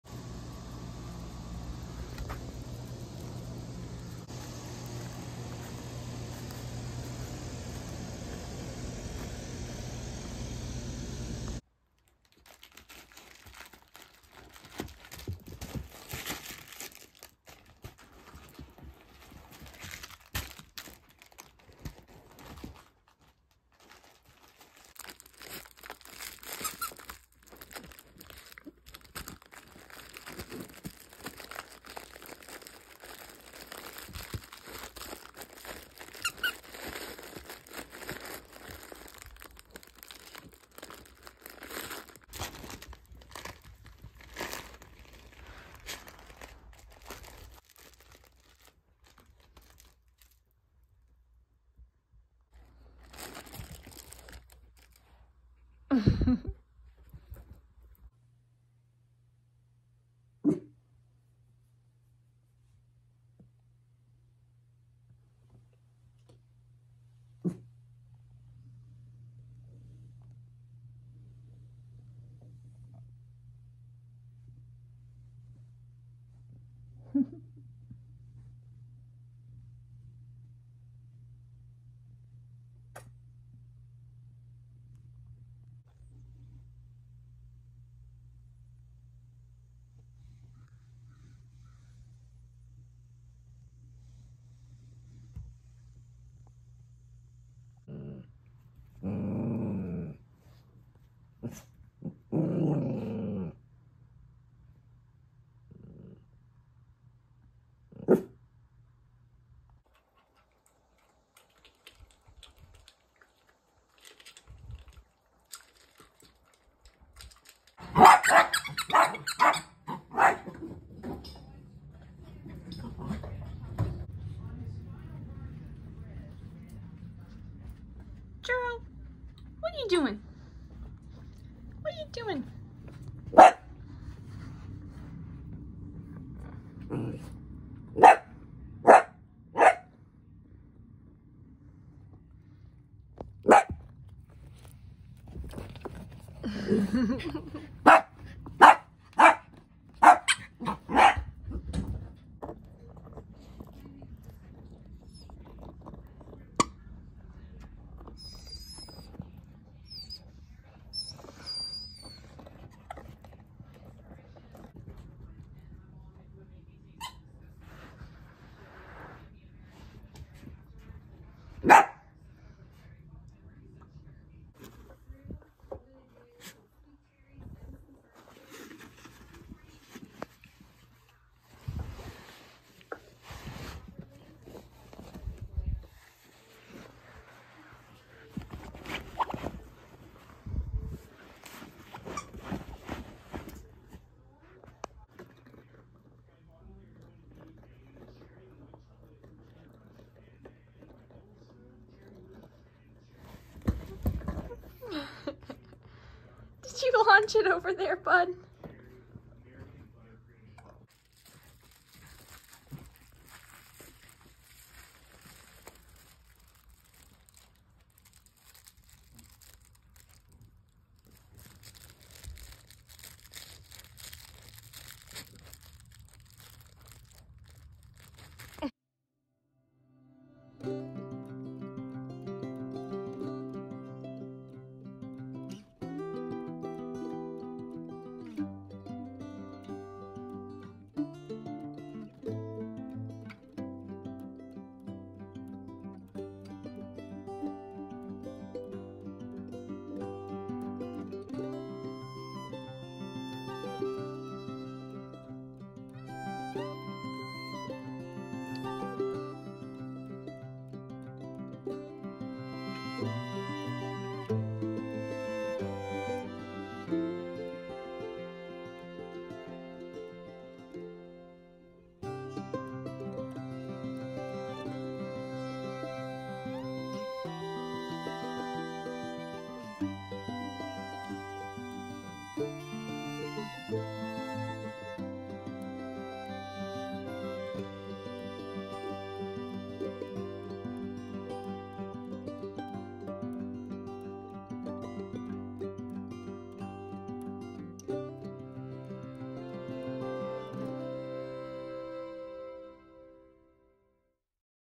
Miniature dachshund puppies playing!
April and Churro living their best miniature dachshund life!